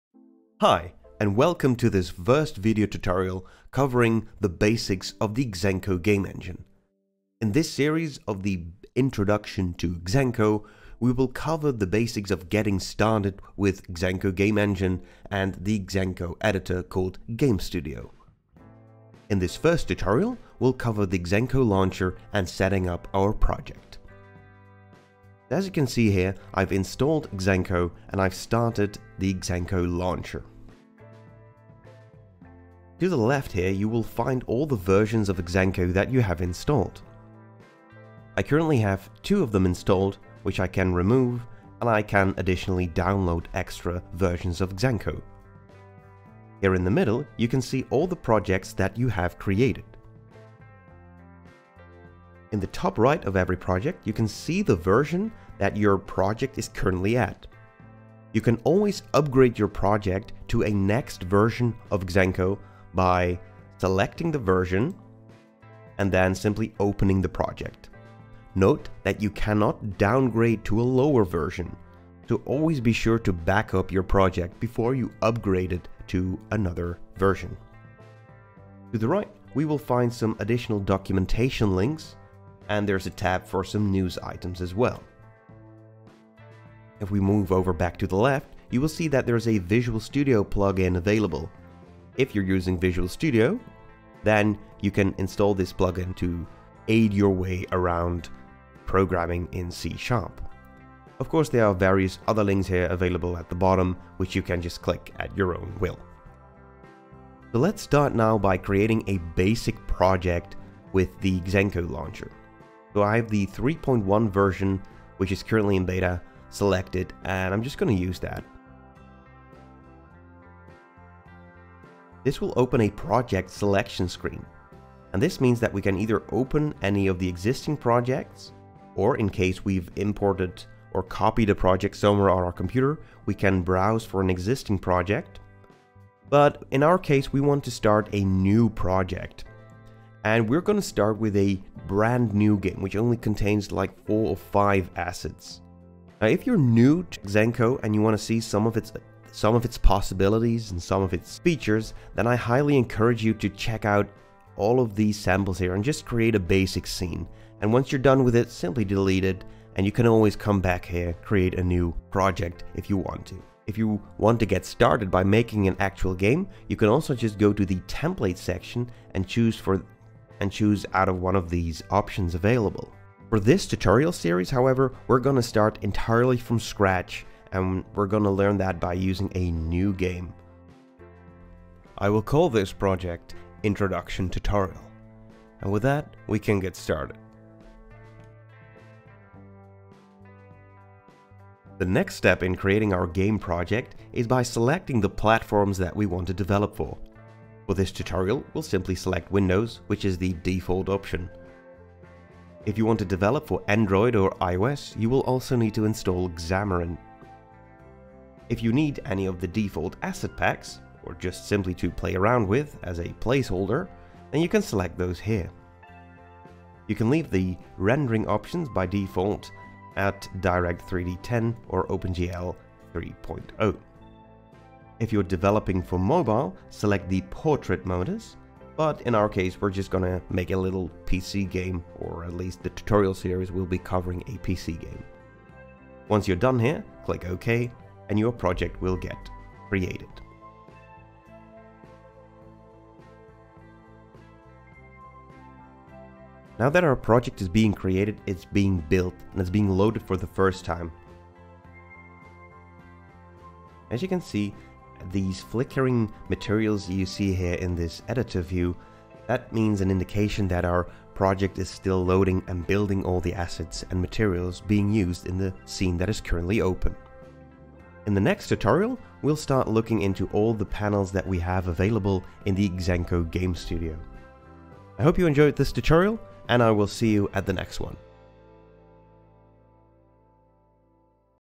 Stride basics #1 - The launcher
This is the first tutorial of a series of tutorials for the Stride game engine. This series covers the basics. Recordings were made when it was still called Xenko.